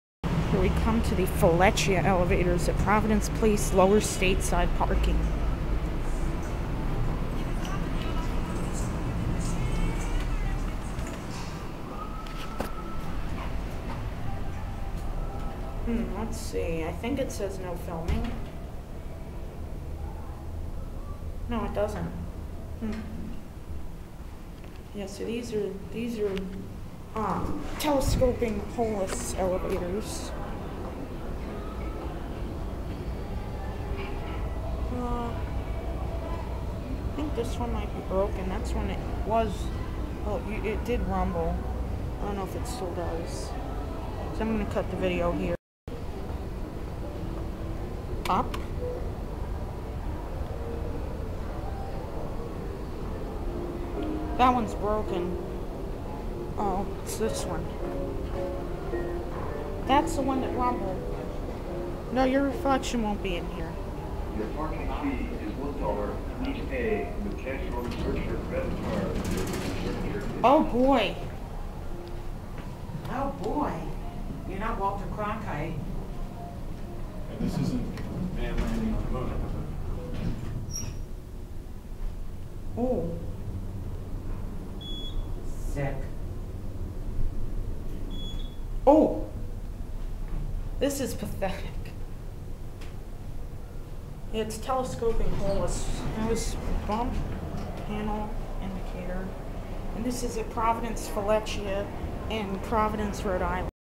Schindler Hydraulic Elevators to Lower Stateside Parking Levels - Providence Place
Technical Specs:
Brand: Schindler
Type: Telescoping Holeless Hydraulic
Speed: 200fpm according to RI State Certificate
Distance: 60ft/4 Floors (*C, B, A, Mall 1)
Year Installed: 1999